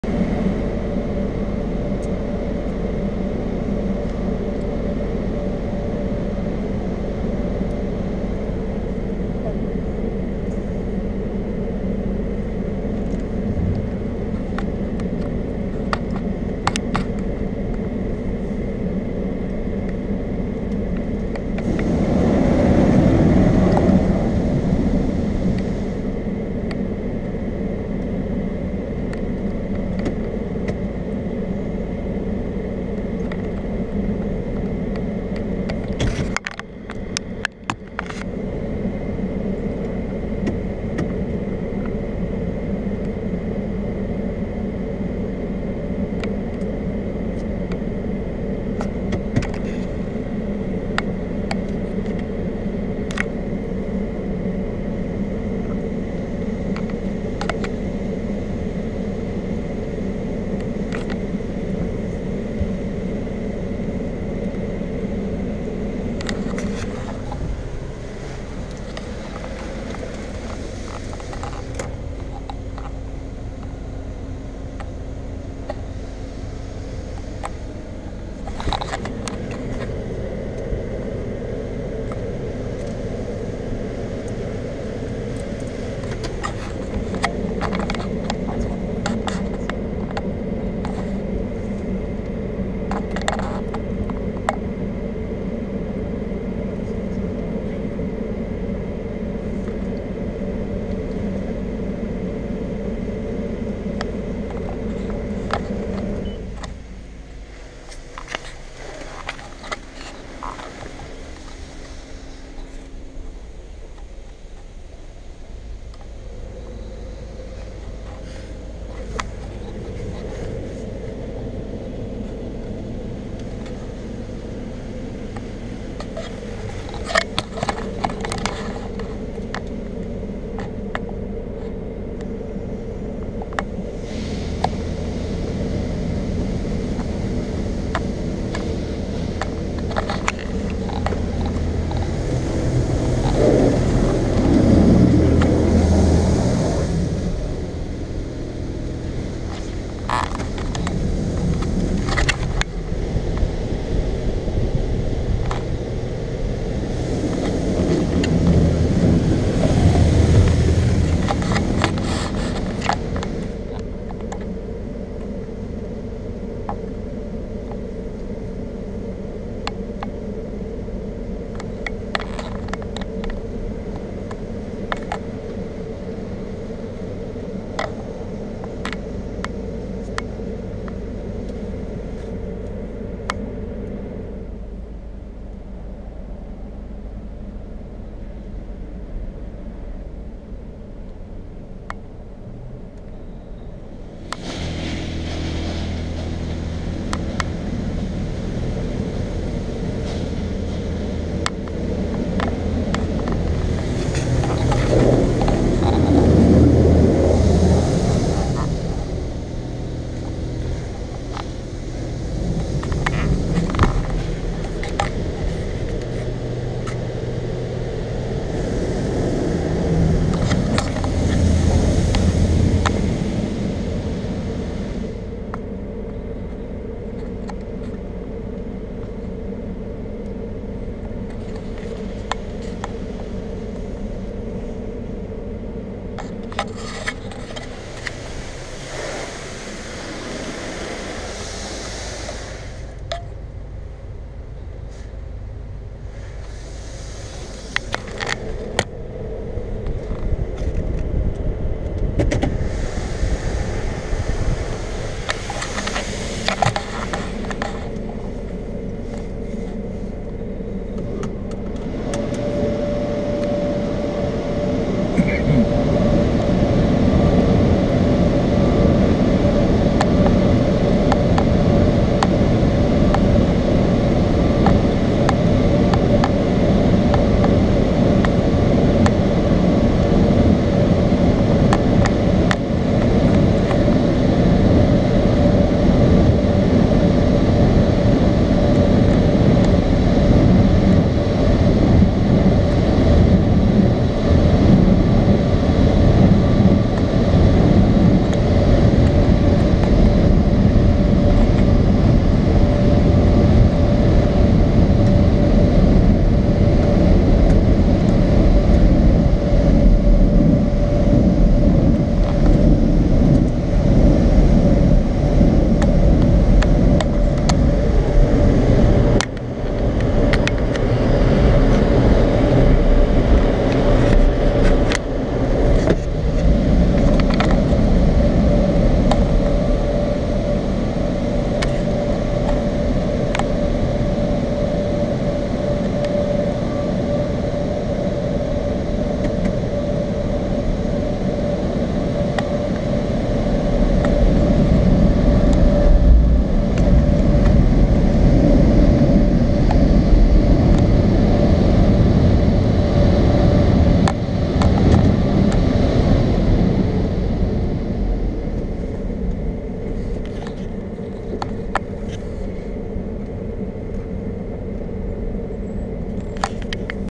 Washworld High Velocity @ Chevron gas station, San Juan Capistrano California 6-20-17
My sisters car needed a wash and we were on Vacation in Dana Point at a condo called Capistrano Surf Side Inn, so I looked on my phone for touchfree car washes near by and I found this High Velocity at a Chevron located on Avery Pkwy and Camino Capistrano in San Juan Capistrano. This was a weird wash because it had to signs in the wash bay ,and had odd looking dryers ,but that doesn't really matter it did a great job for $10.